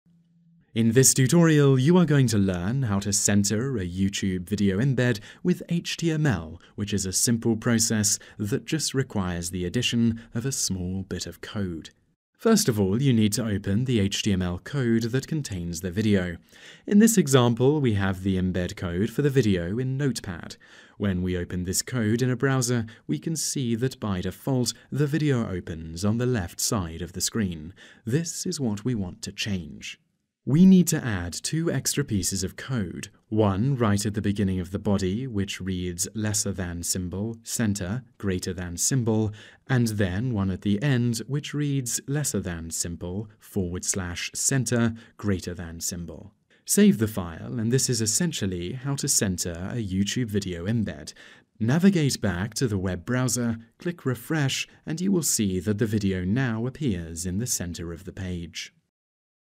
How to Center YouTube Video Embed
In this tutorial you will learn how to center an embedded YouTube video using HTML.

In this tutorial you are going to learn how to center youtube video embed with HTML which is a simple process that just requires the addition of a small bit of code.

Step # 1 -- Viewing the Current Alignment

First of all you need to open the HTML code that contains the video. In this example we have the embed code for the video in Notepad. When we open this code in a browser we can see that by default the video appears on the left of the screen -- this is what we want to change.

Step # 2 -- Inserting the Extra Code

We need to add two extra pieces of code, one right at the beginning of the body which reads "center" and then one at the end which reads "/center". Save the file and this is essentially how to center youtube video embed, navigate back to the web browser, click refresh and you will see that the video now appears in the center of the page.